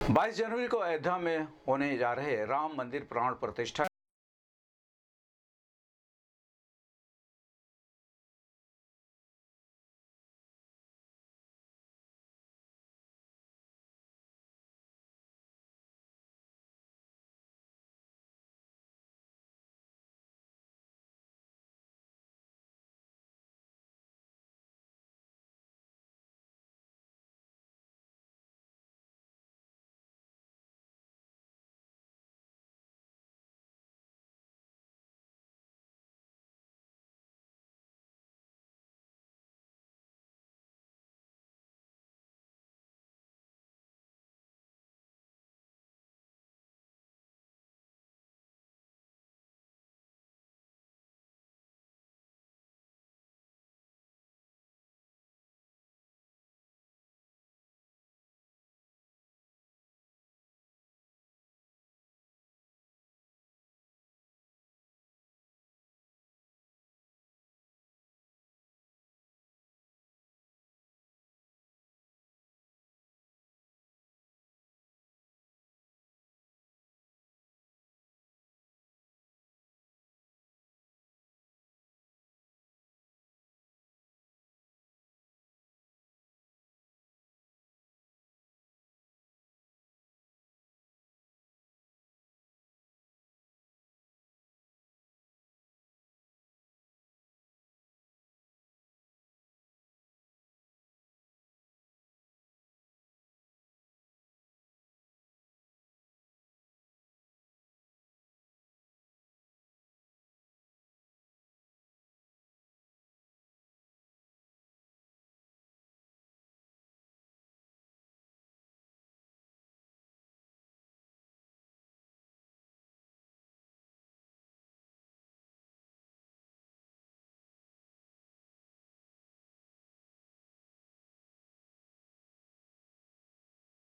सीएमओ डॉक्टर आशुतोष कुमार दुबे द्वारा चलाया गया स्वच्छता अभियान
शासन के निर्देशानुसार सीएमओ डॉक्टर आशुतोष कुमार दुबे द्वारा अपने अधीनस्थ अधिकारियों और कर्मचारी गण के साथ चलाया गया स्वच्छता अभियान, अस्पताल परिसर व मंदिर के साफ-सफाई कर स्वच्छता की जलाई अलख